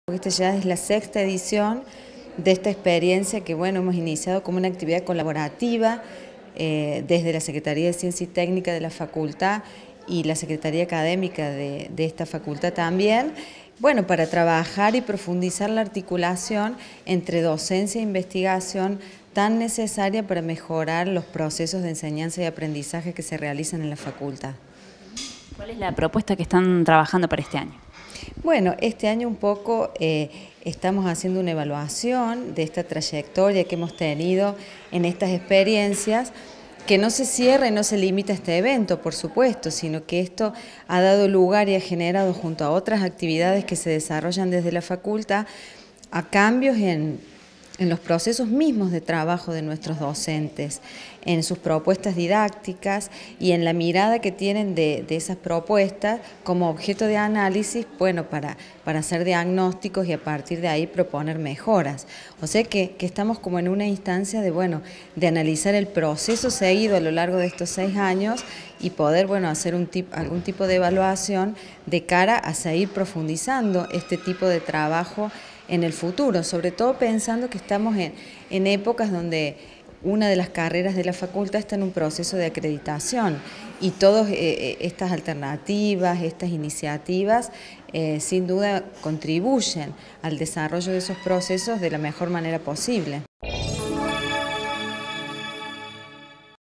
COMENZARON LAS 6º JORNADAS SOBRE INVESTIGACIONES Y EXPERIENCIAS EDUCATIVAS EN ECONÓMICAS
CECILIA FICCO – SECRETARIA CIENCIA Y TÉCNICA FCE
Facebook.com/uniriotv
Twitter.com/uniriotv